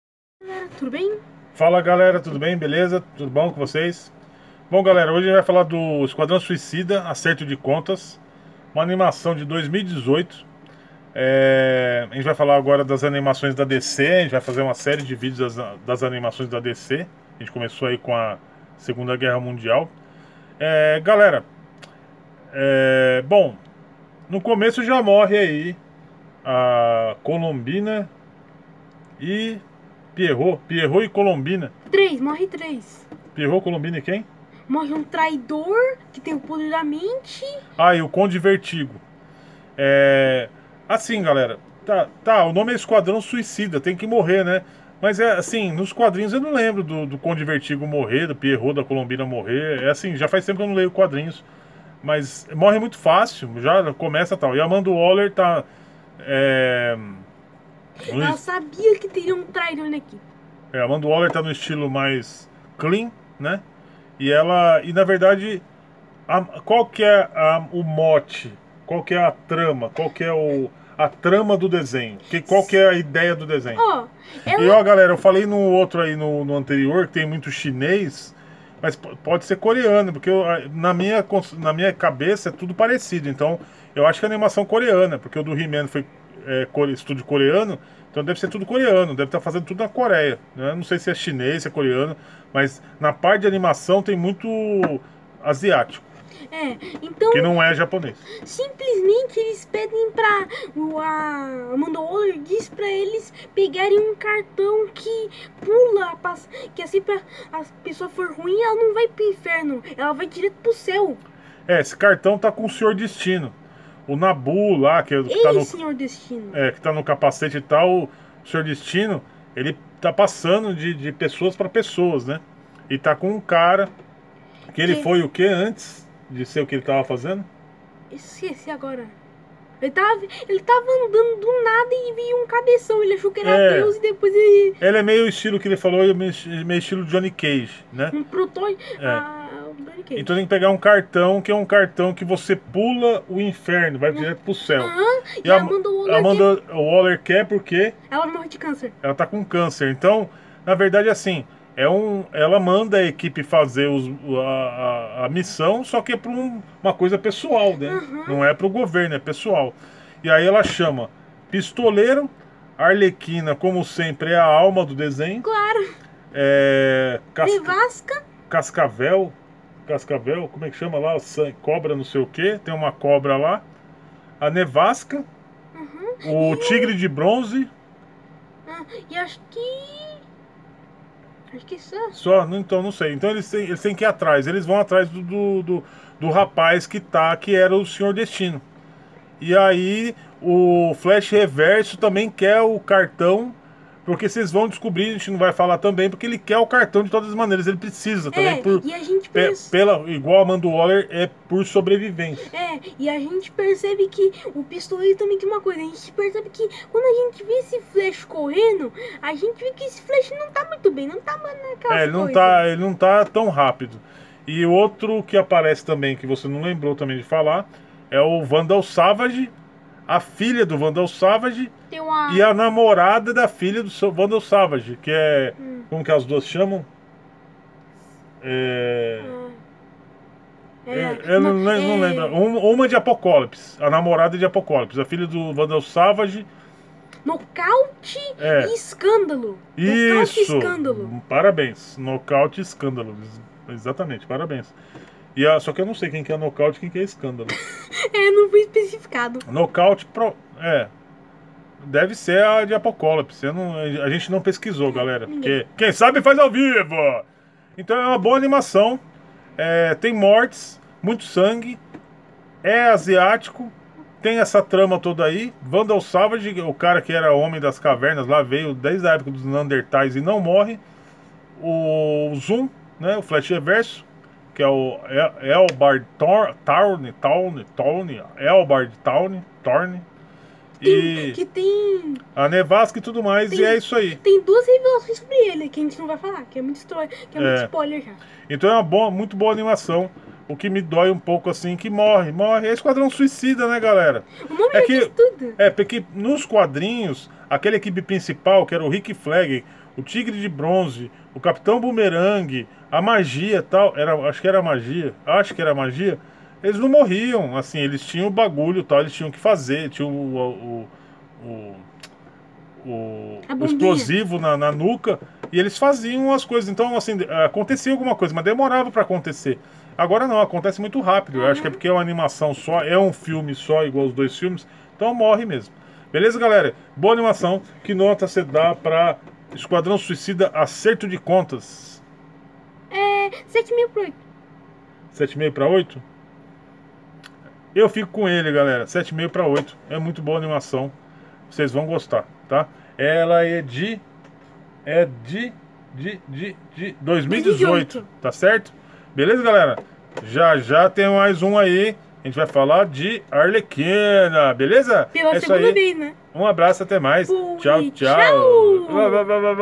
Esquadrão Suicida - Acerto de Contas - o que achamos da animação
O que achamos de Esquadrão Suicida - Acerto de Contas, animação da DC Comics estrelada pela Força-Tarefa X. Considerações certeiras e rasteiras. É rapidinho! É vapt-vupt!

- Blog com novidades diárias sobre quadrinhos